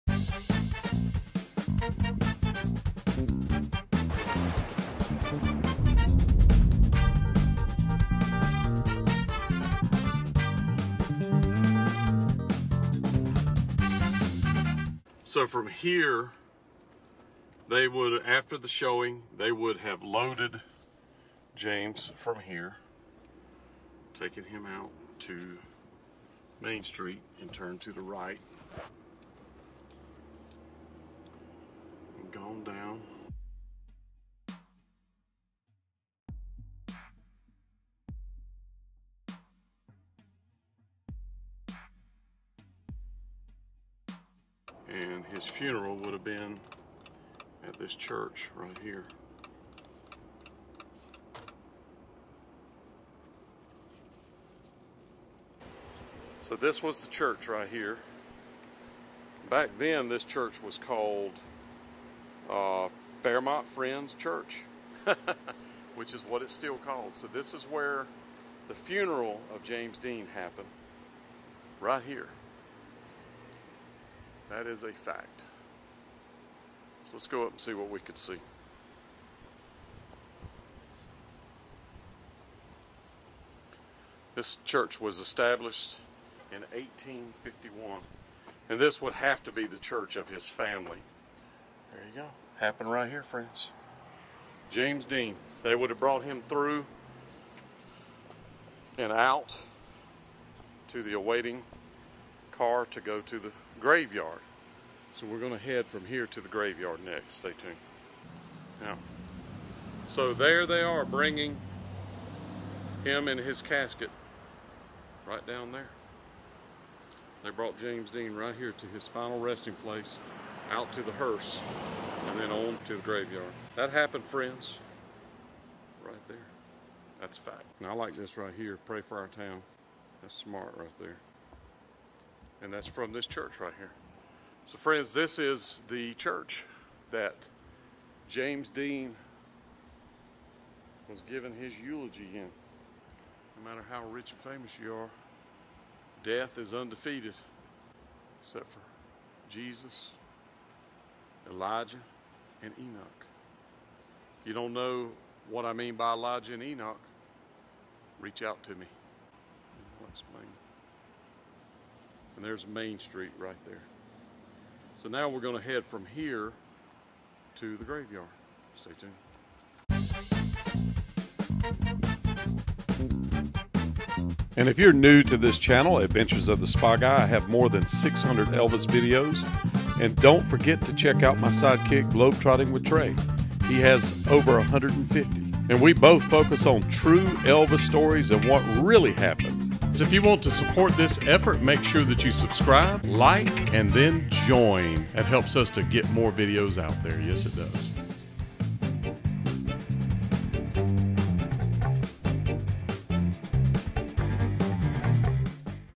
Episode #4 James Dean Series Friends Church Funeral Was Held Here..
In this video The Spa Guy takes you to the Area of Indiana where he was born and raised for the biggest part of his life. Fairmont and Marion.. We explore and do photo line ups of the many photos taken by Dennis Stock and others was well as the Down Town, High School, Funeral Home, Funeral Church, Cemetery and Grave, Motorcycle, Shop, Home Place, Birth Home Spot and his Mother's Grave..  Like, Share, Join and Subscribe..